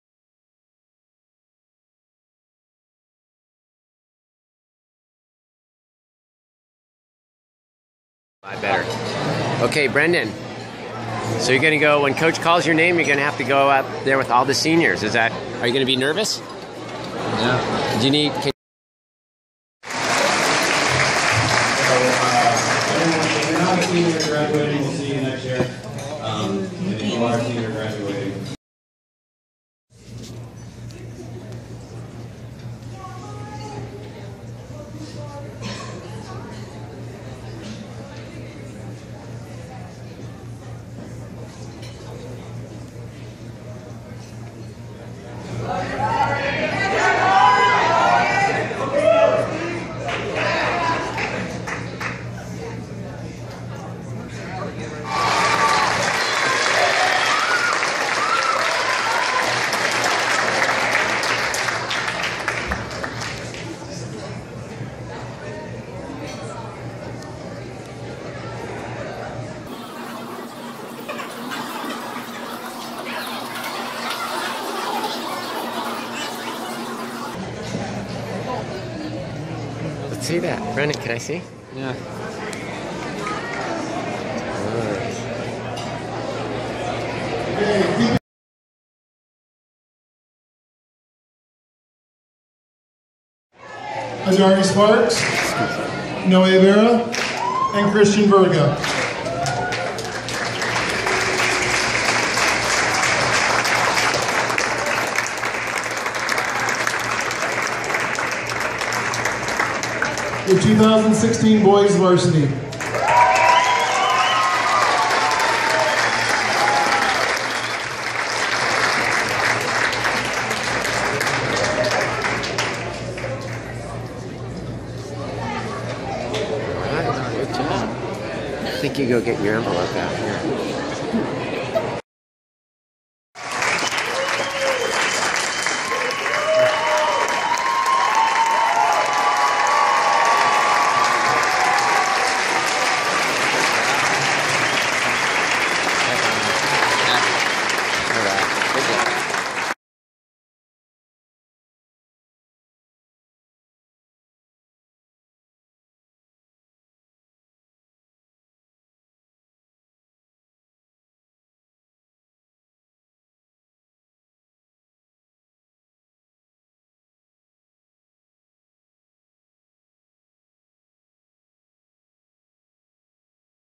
Brendan's track banquet 2016
The Los Alamitos HS track banquet 5/24/16.  This is my 2nd banquet. Coach Howard seems to give out more academic awards than track awards at these events.